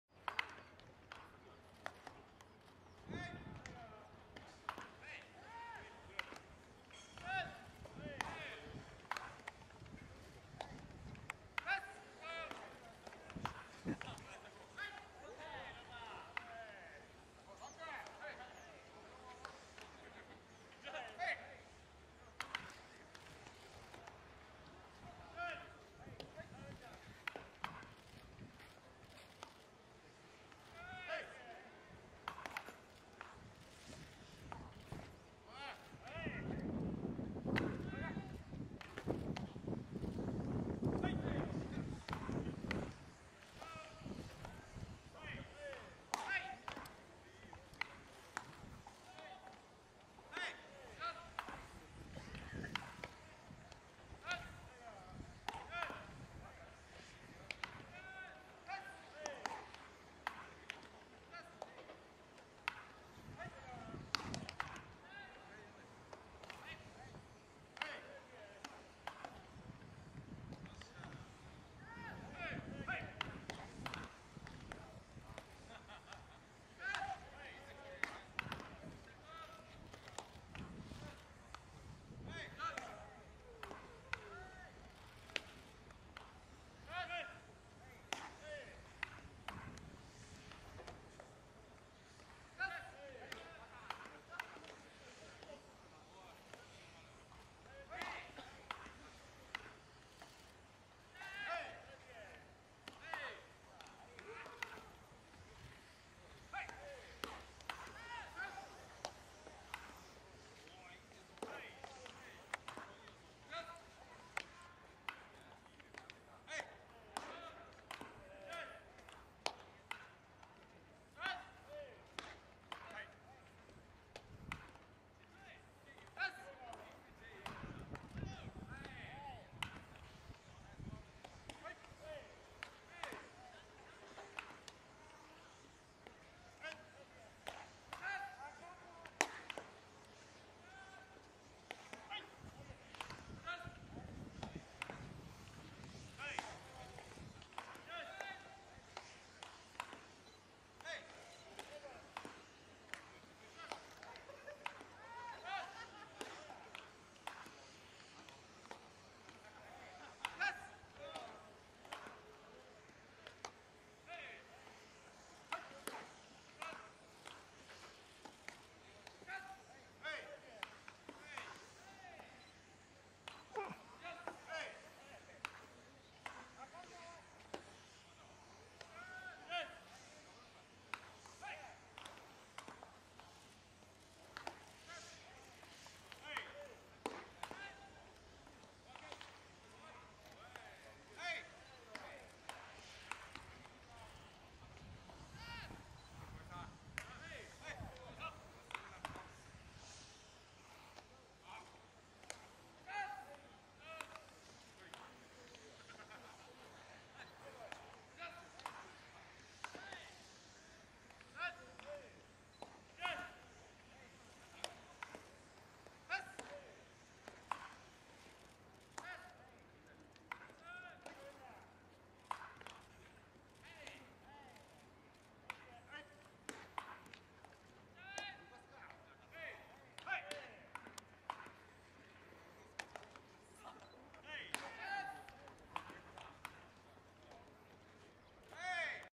2010/1/30 自主トレ 野手陣へのノック
2010/1/30 中日ドラゴンズ 自主トレ @ 北谷球場
野手陣へのノック